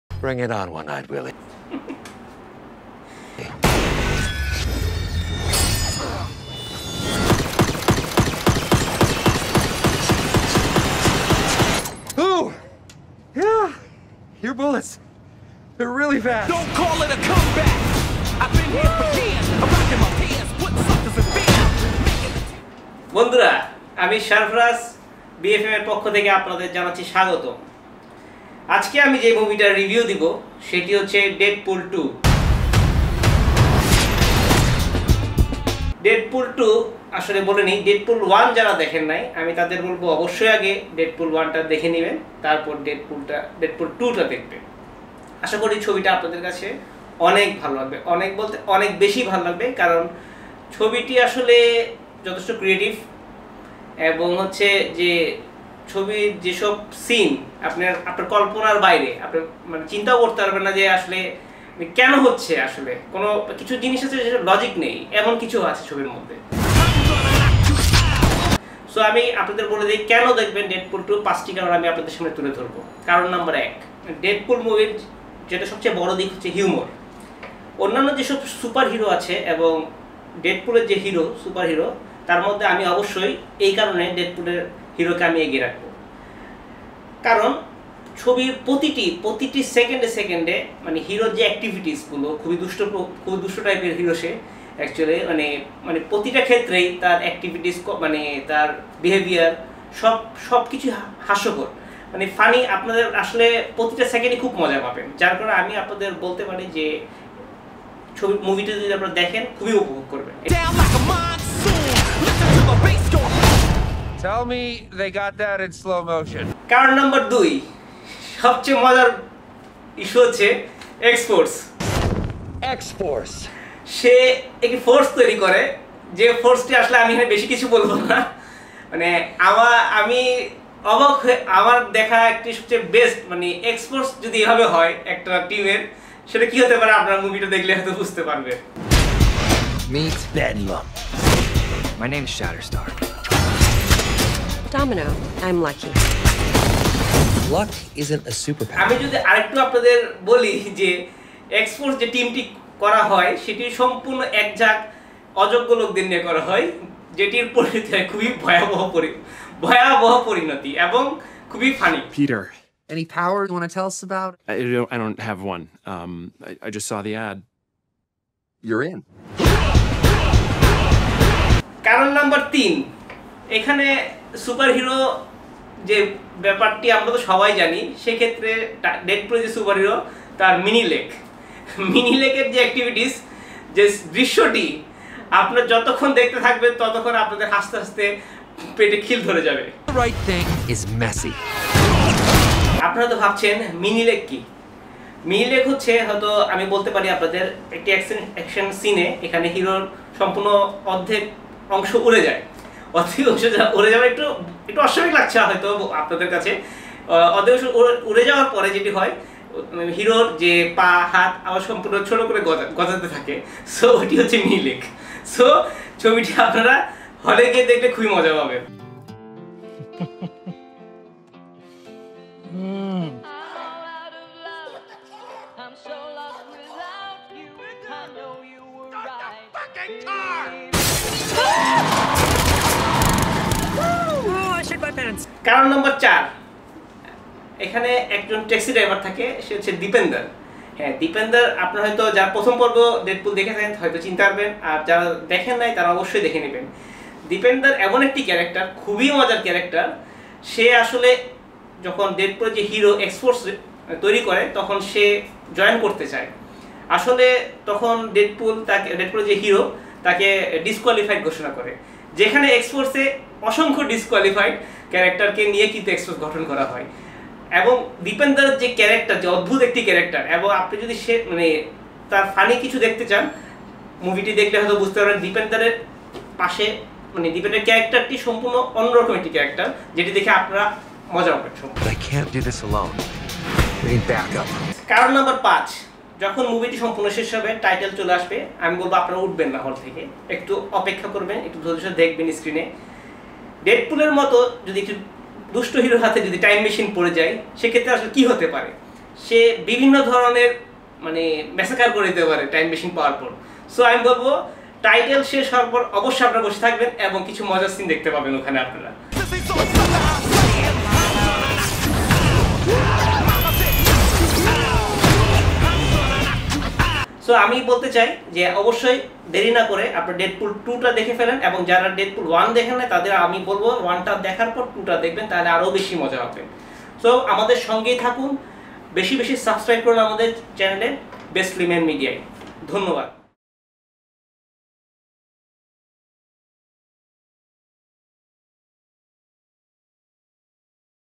যে ৫ টি কারণে ”ডেডপুল ২” মুভিটি দেখবেন - DeadPool 2 Movie | 5 Reasons to Watch
Host: Sarfaraz Rana
Video: Sohel Afgani RaNA